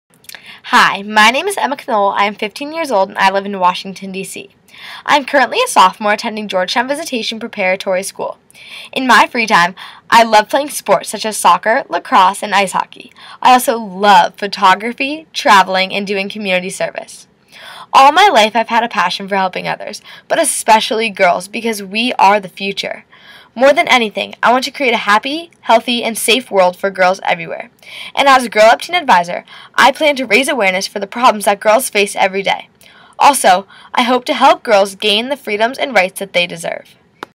Teen Advisor Emma Knoll
Hi! My name is Emma Knoll, and I am 15 years old.  I have lived in Washington, D.C. my whole life, and I am a sophomore at Georgetown Visitation Preparatory School. I have always dreamed that one day this world will be a safe and happy place for girls everywhere, and I hope to help make this dream come true by working with Girl Up.